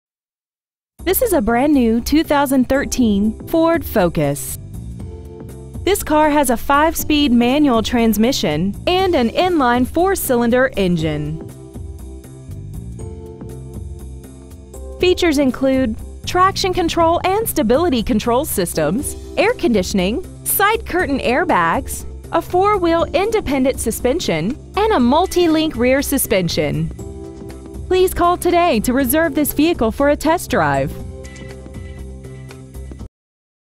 2013 FORD FOCUS Bourbonnais IL
We have been honored to serve the Bourbonnais IL area , we promise that your experience at our dealership will exceed your expectations ! Year : 2013 Make : FORD Model : FOCUS Engine : 2.0L I4 16V GDI DOHC FLEXIBLE FUEL Trans . : 5-SPEED MANUAL Exterior : INGOT SILVER METALLIC Miles : 136 Interior : CHARCOAL BLACK Stock : C13714 Court Street Ford Inc Latham Drive Bourbonnais , IL 60914